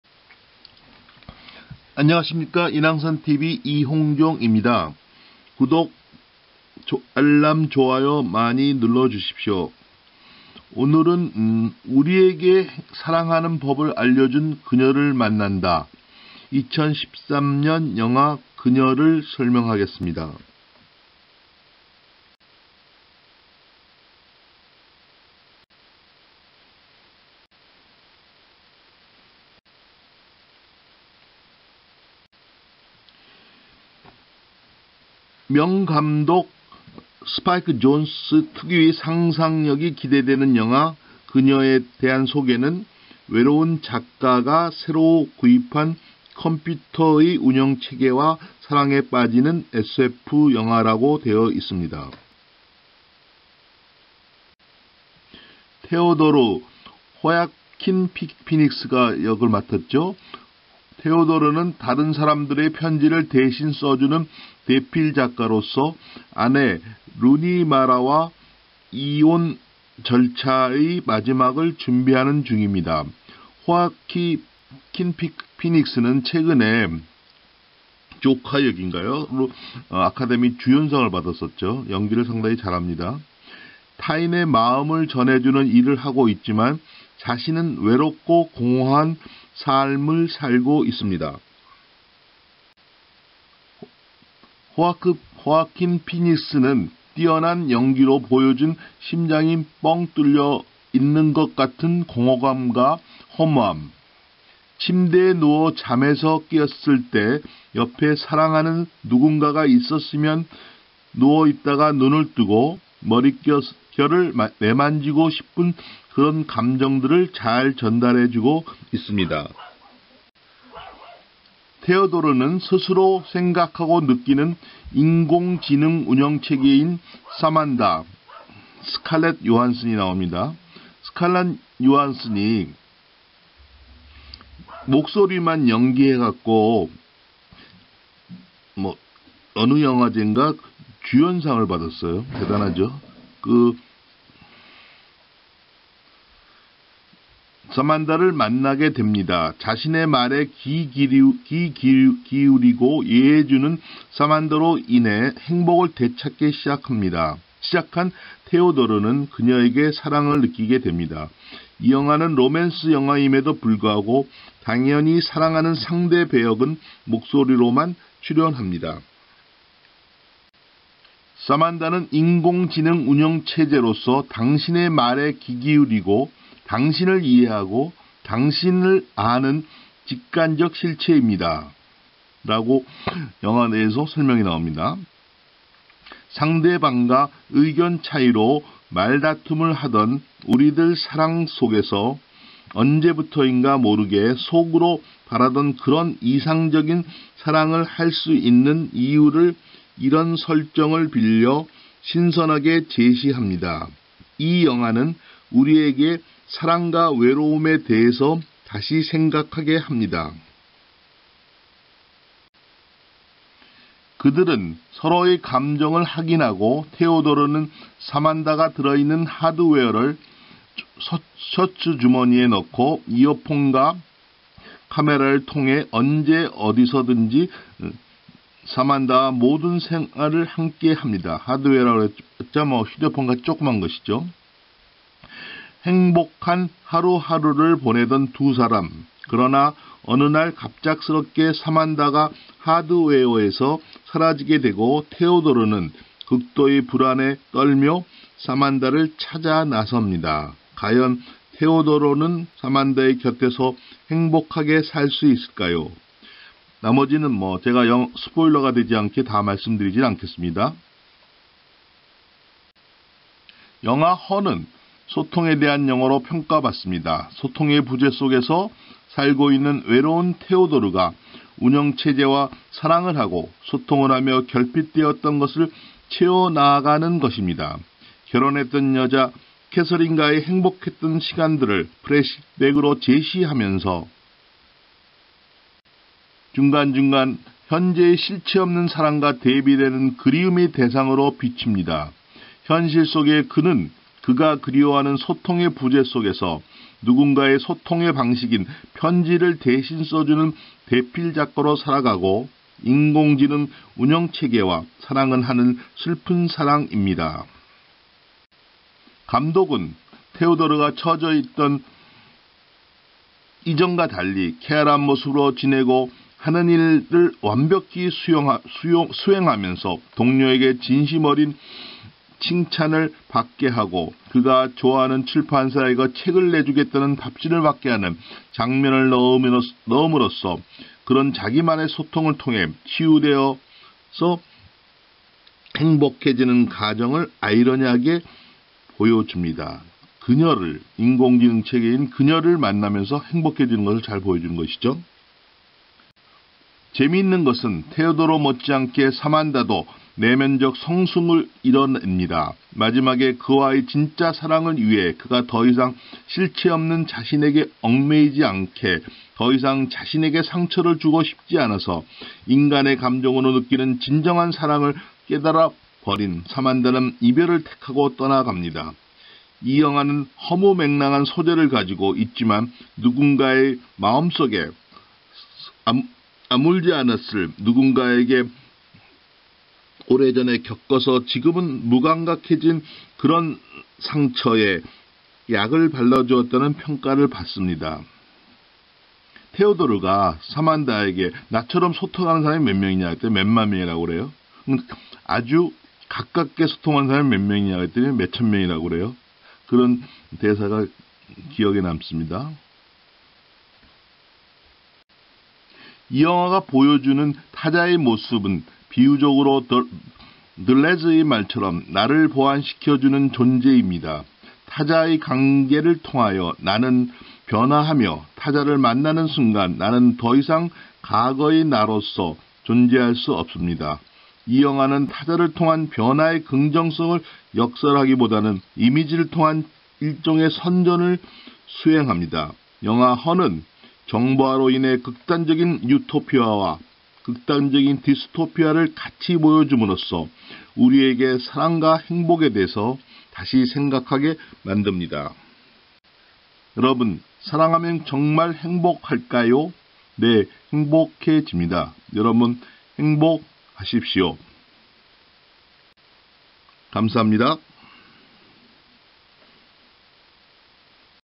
"우리에게 사랑하는 법을 알려준 『그녀』를 만난다." - 〈인왕산TV 이홍종〉 행복학(VI)
여러분! 사랑하면 정말 행복할까요? 네, 행복해집니다! 여러분! 행복하십시요!!